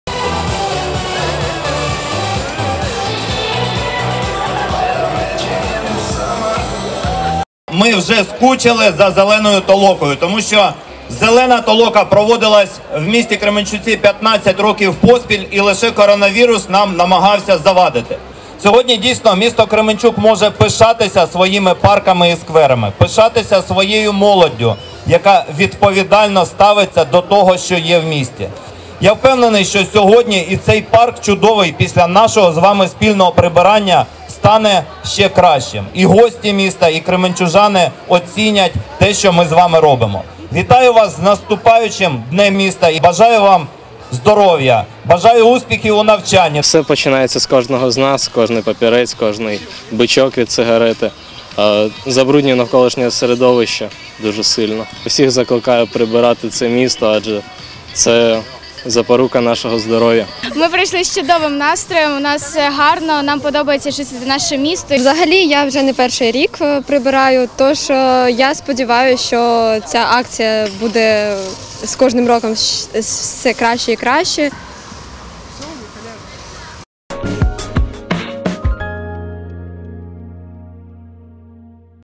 Екологічна акція «Зелена толока» зібрала 1700 учасників, школярів та студентів міста
Екологічна акція «Зелена толока» зібрала 1700 учасників, школярів та студентів міста. Молодь гуртом прибирала парк «Ювілейний»